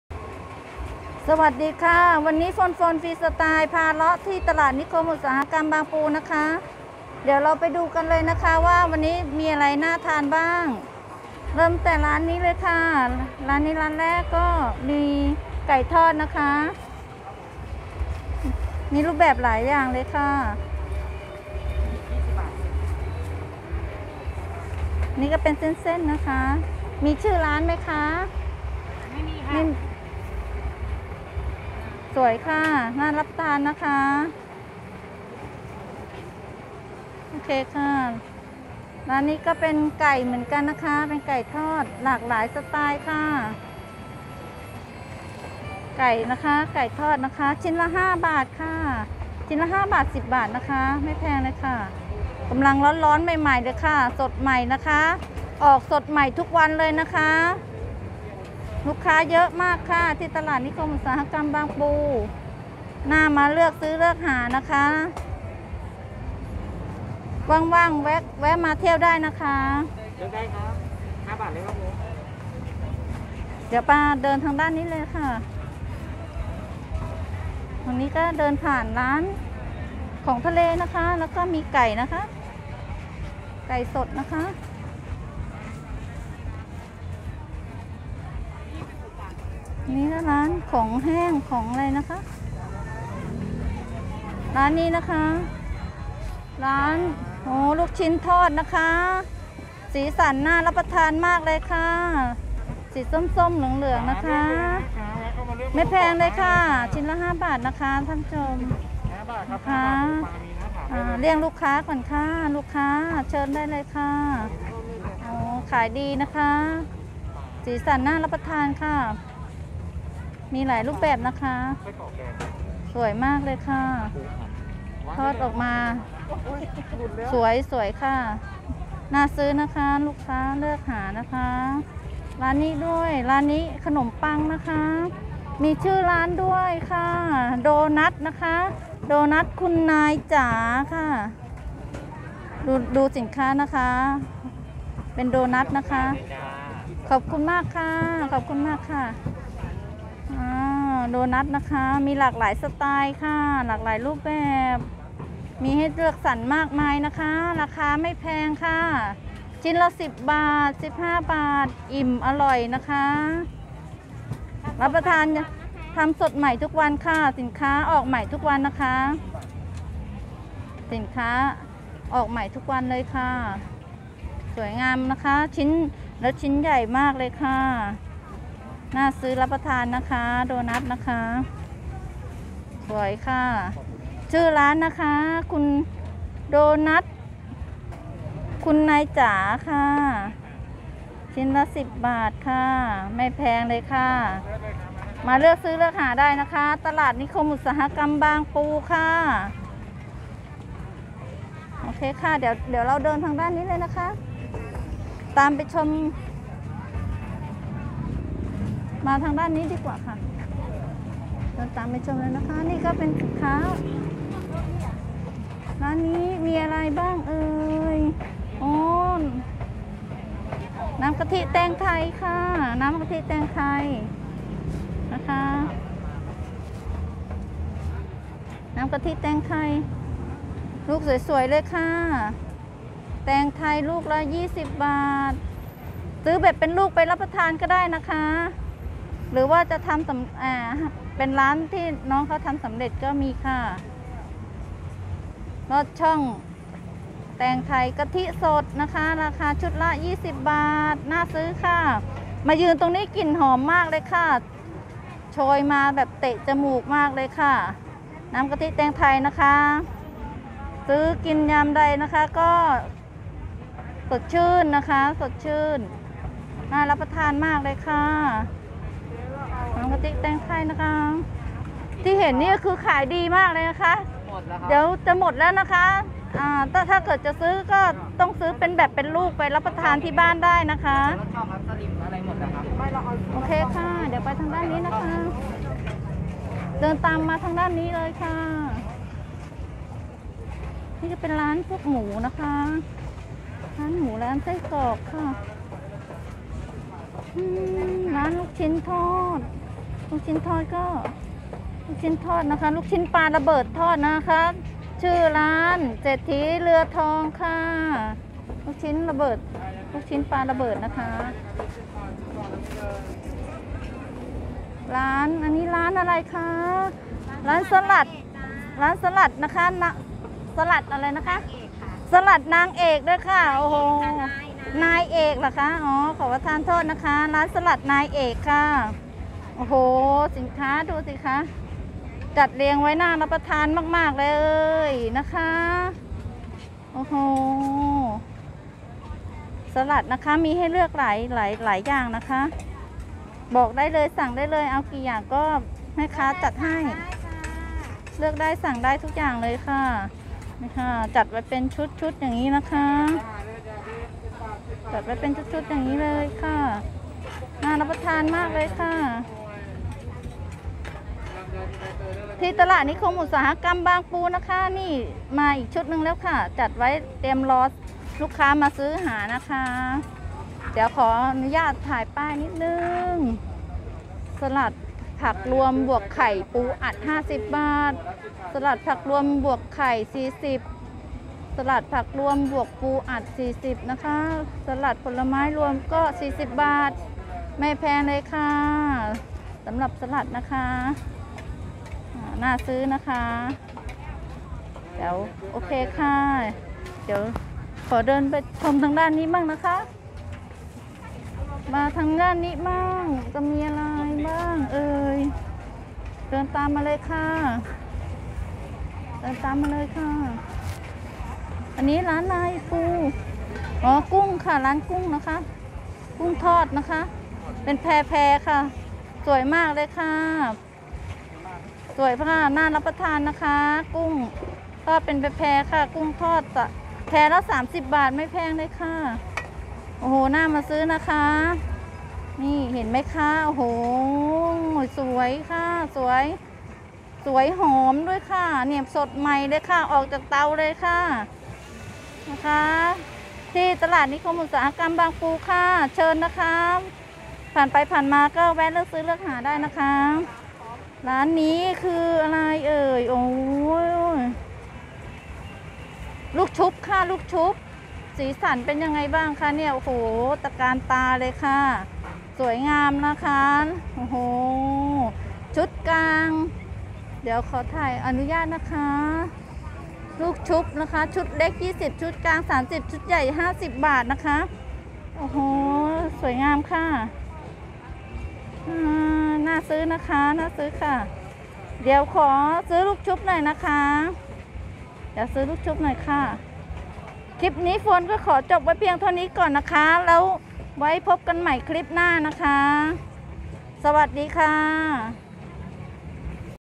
เลาะอีกแล้ว#ตลาดนิคมอุตสาหกรรมบางปูตลาดนัด
พาเลาะเดินชมสินค้าที่ตลาดนิคมอุตสาหกรรมบางปู มีสินค้า อุปโภค บริโภค ให้เลือกสรรมากมาย มีสินค้ามากกว่า 500 ชนิด สินค้าบริโภคสดใหม่ทุกวัน มีสินค้าสดเช่น อาหารทะเล กุ้งสด หมึกสด อาหารปิ้งย่างสดใหม่จากเตา ขนมปังโดนัททำออกใหม่สดๆทุกครั้งที่ออกตั้งร้าน ลูกชิ้นปิ้ง ลูกชิ้นทอดออกจากเตาร้อนๆใหม่ๆ กุ้งมอดปูทอด ไก่ทอด นื้อไก่ทอดต่างๆมากมาย ลูกชุปสีสันน่าทานมากๆสดใหม่ น้ำกะทิแตงไทย ลอดช่อง นี่เป็นร้านค้าส่วนหนึ่ง ของร้านค้าที่ตลาดนิคมอุตสาหกรรมบางปู ซึ่งมีร้านค้ามากกว่า 300 ร้านค้า มาแวะเที่ยวชม เลือกซื้อเลือกหาได้ค่ะ น่าเดินมากๆค่ะ ตลาดที่นี่จะมีทุกวันค่ะไม่มีวันหยุดนะคะ มาได้ทุกวันสินค้ารอบริการทุกท่านแล้วค่ะ